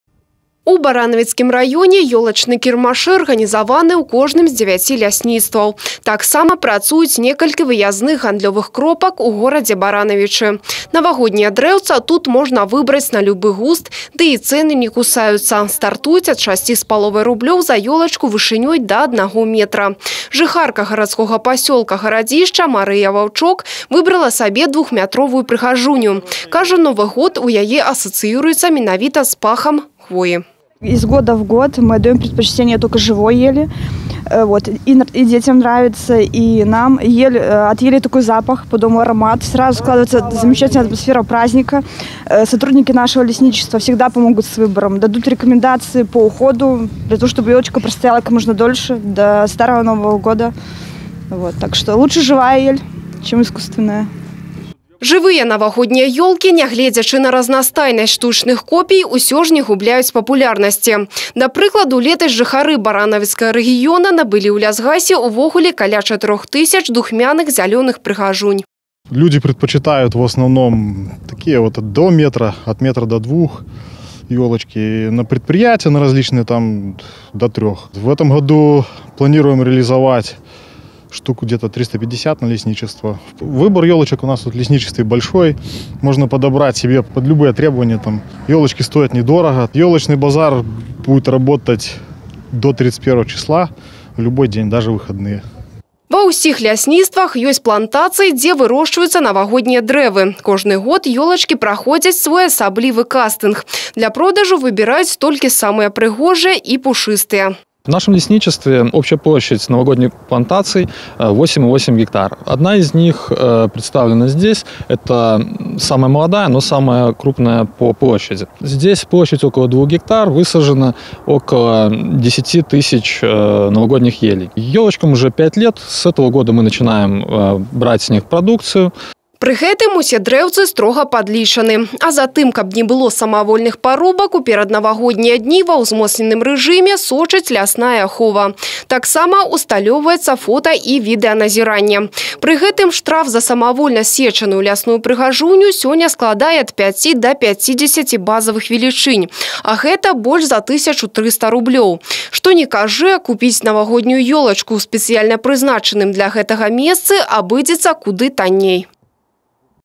НАВАГОДНІЯ ПРЫГАЖУНІ (Відэа)
Доўгачаканыя навагоднія святы ўжо не за гарамі. А значыць, набліжаецца час для таго, каб набыць зялёную лясную прыгажуню. Ёлачныя кірмашы ужо пачалі працаваць па ўсёй Брэстчыне, у вялікіх і малых населеных пунктах.